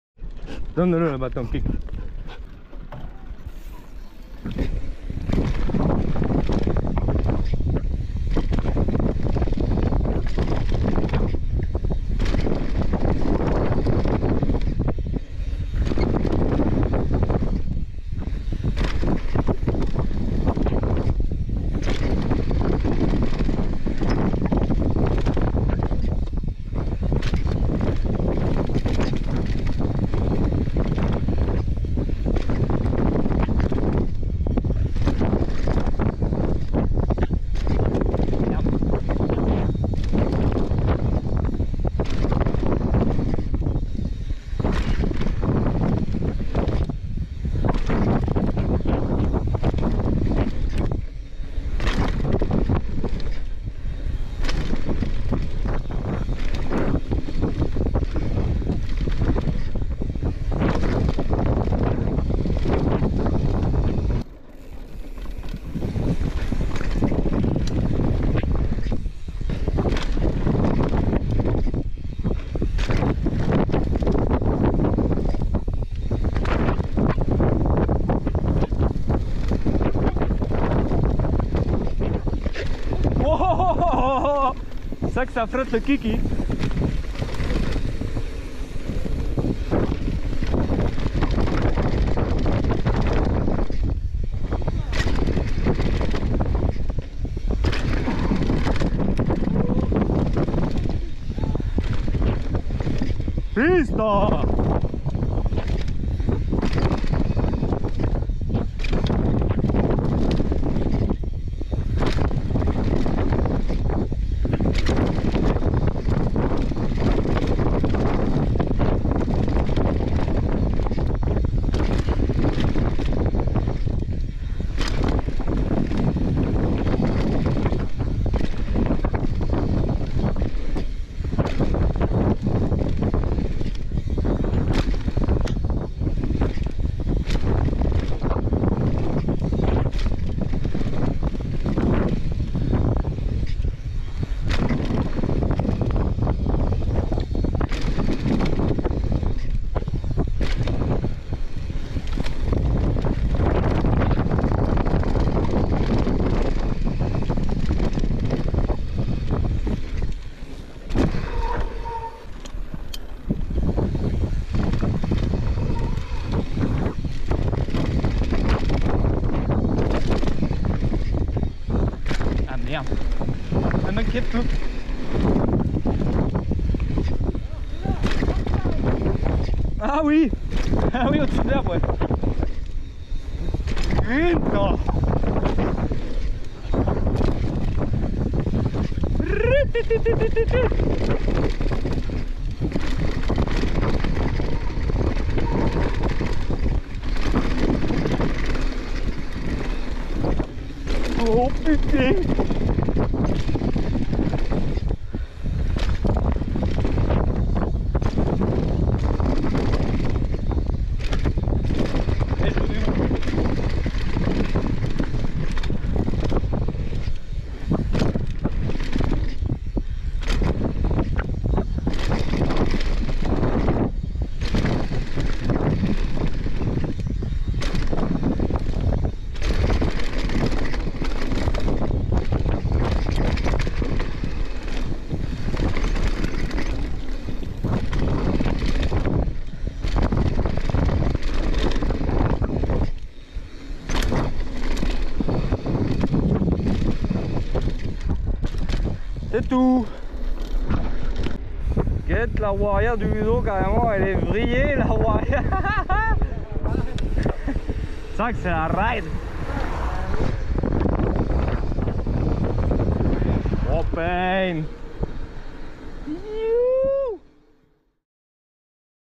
WHIP IT avec le Ludg
Dernière session des deux jours passé à l’EVO bike park.
Sur cette vidéo nous roulons la whip it. Il m’aurai fallu un jour de plus pour la rouler correctement. Vous aller voir sa accroche parfois.